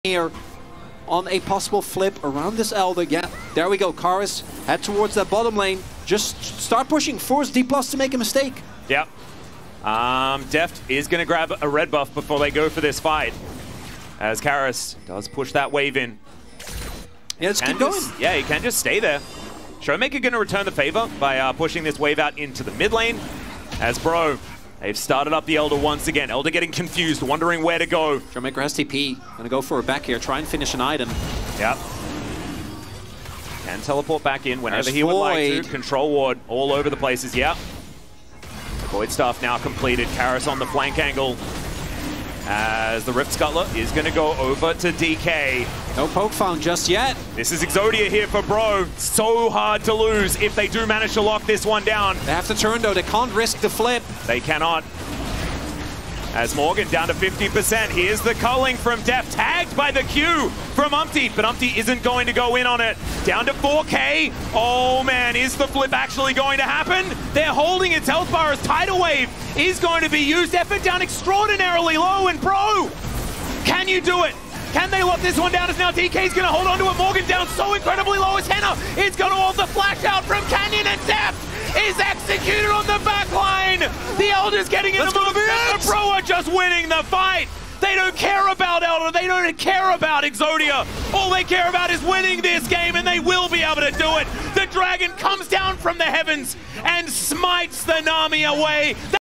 Karis executes Deft in a game winning moment!! Elder dragon fight! DK Vs BRO game1
Great team fight for elder dragon. Karis played well to execute Deft.